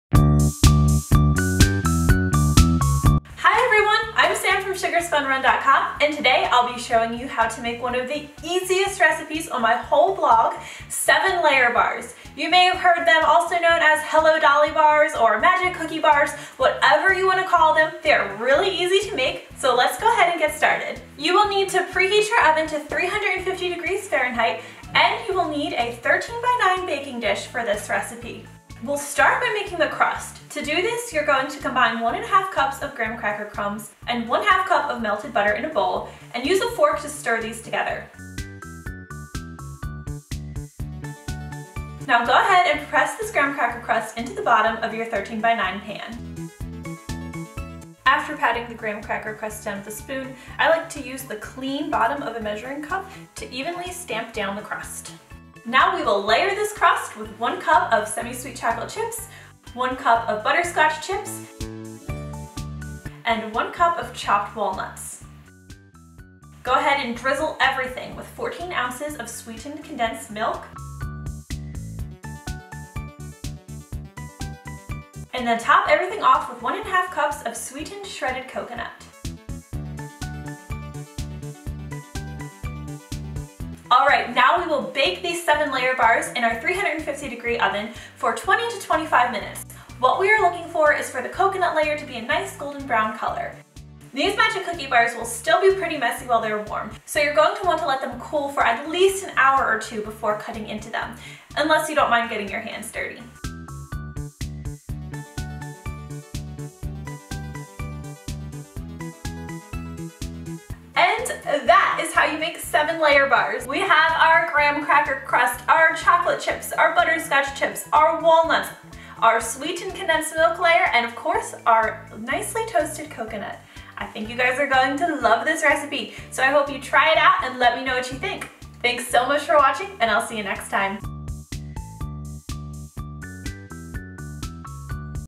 How to Make 7 Layer Bars
A quick and simple guide to making 7 Layer Bars at home!  These are so easy with just 7 ingredients! Also known as "Magic Cookie Bars" or "Hello Dolly Bars".

Ingredients
1 ½ cups graham cracker crumbs
½ cup salted butter melted (113g)
1 cup semisweet chocolate chips (170g)
1 cup butterscotch chips (170g)
1 cup chopped walnuts or pecans
14 oz sweetened condensed milk (395g)
1 1/2 cups sweetened shredded coconut

Instructions
00:00 Introduction

00:20  Preheat your oven to 350F (175C) and lightly butter the sides and bottom or a 9x13 baking dish.  Set aside.

00:28  Combine graham cracker crumbs and melted butter butter in a bowl and use a fork to stir until well-combined.  Pour into the bottom of your prepared pan and use the clean bottom of a measuring cup to tamp into the bottom of pan

00:55  Pour chocolate chips, then butterscotch chips, then chopped nuts, evenly over the graham cracker crust layer.

01:06  Drizzle ingredients in pan evenly with sweetened condensed milk.

01:14  Evenly sprinkle coconut over the condensed milk.

01:23  Transfer to oven and bake 350F for 25 minutes or until coconut is turning lightly golden brown.
Allow bars to cool several hours before cutting -- this will allow the chocolate to firm up and make them easier to cut.  If you cut them sooner they'll be quite messy (but still delicious!)